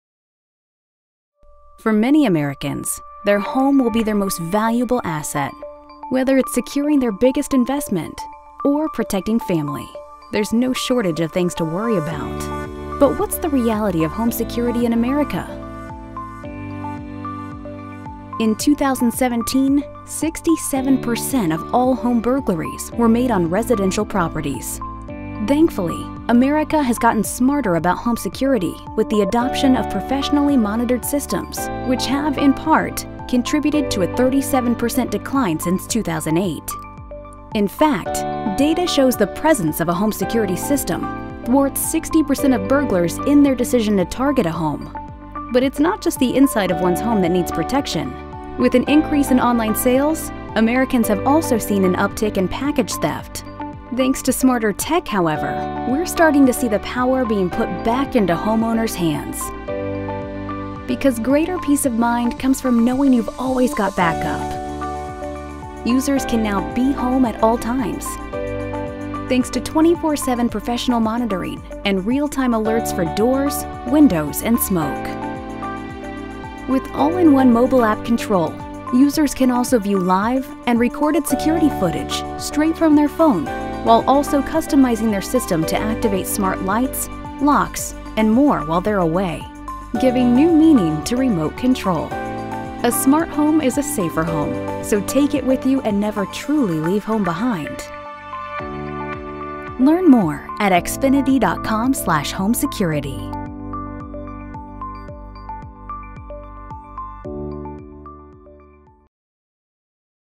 The State of Home Security in America
Data shows the presence of a home security system thwarts 60% of burglars in their decision to target a home. Learn how smart home tech can help protect what matters most.